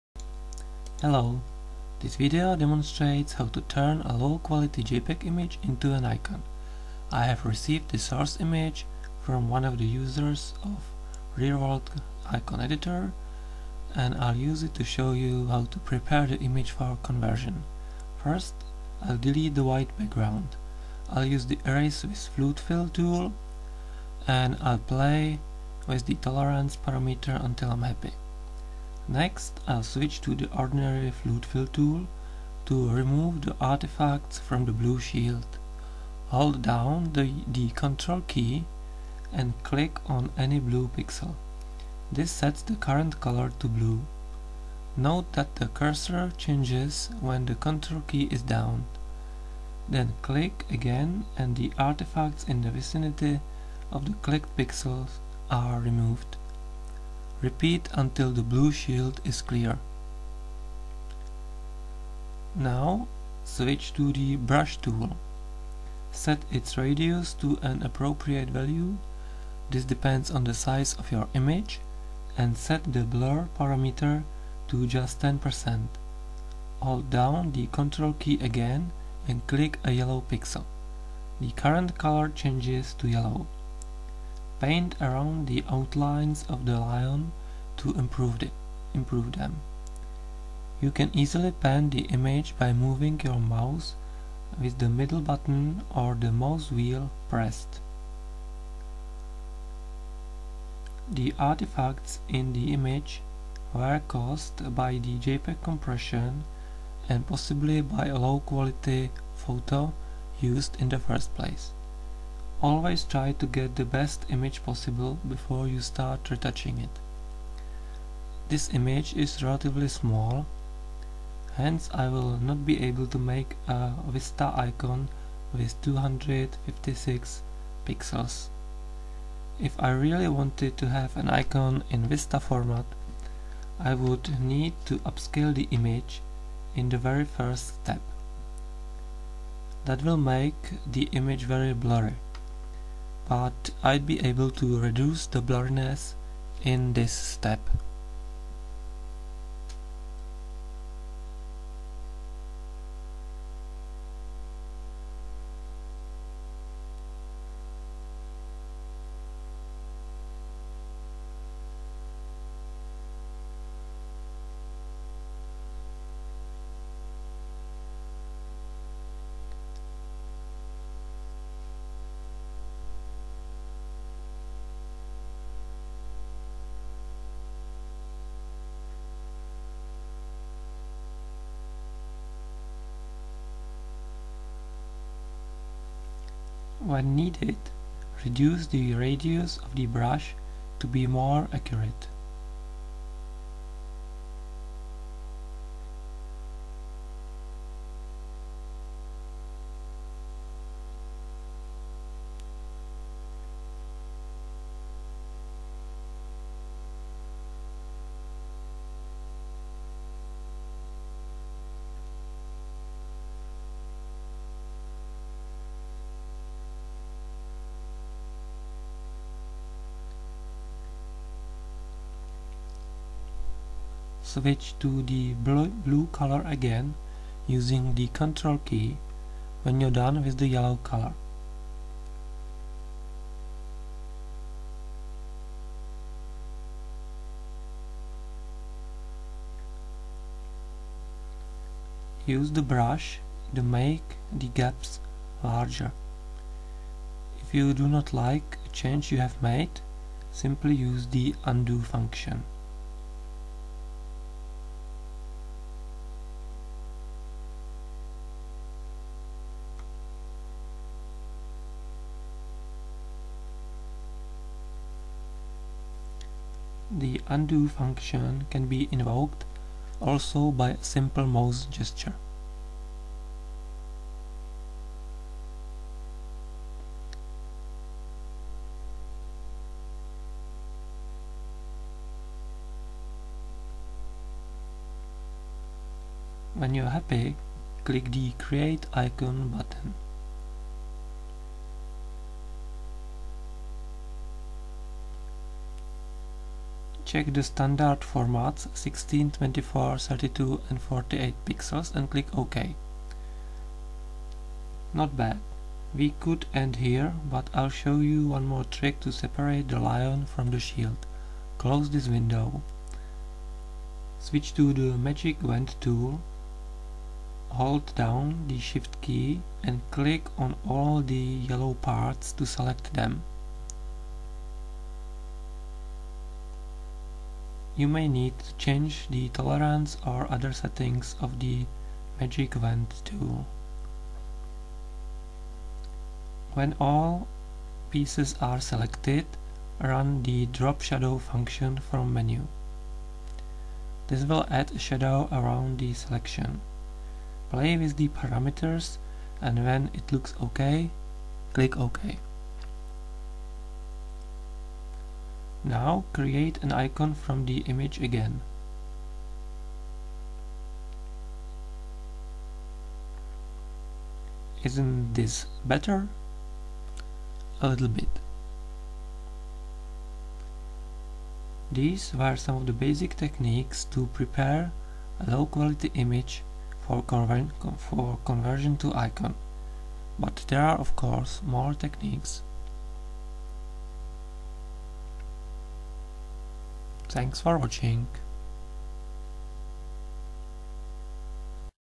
Make icon from a blurry jpg image
Learn how to create icons from low quality images in RealWorld Icon Editor